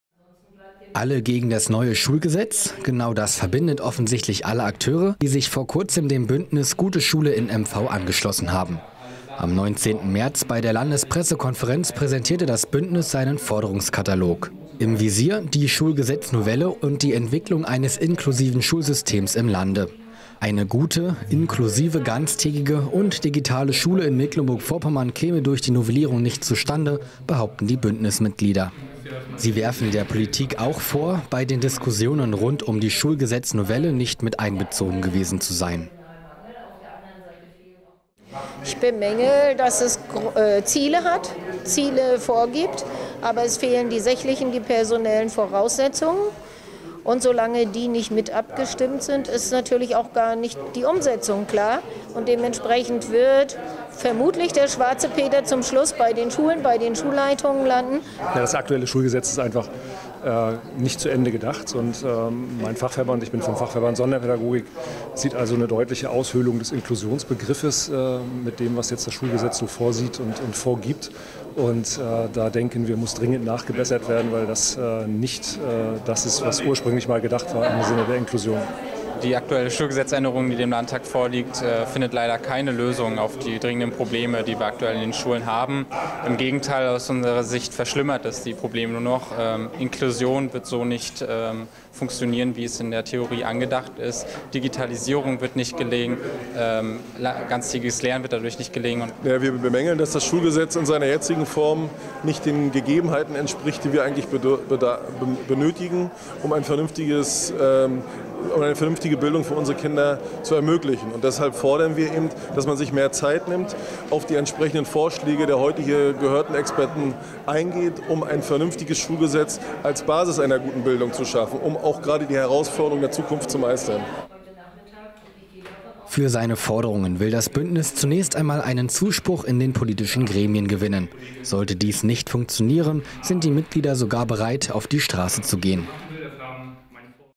Alle gegen das neue Schulgesetz?
Genau das verbindet offensichtlich alle Akteure, die sich vor Kurzem dem Bündnis „Gute Schule in MV“ angeschlossen haben. Am 19. März bei der Landespressekonferenz präsentierte der Bündnis seinen Forderungskatalog. Im Visier – die Schulgesetznovelle und die Entwicklung eines inklusiven Schulsystems im Lande.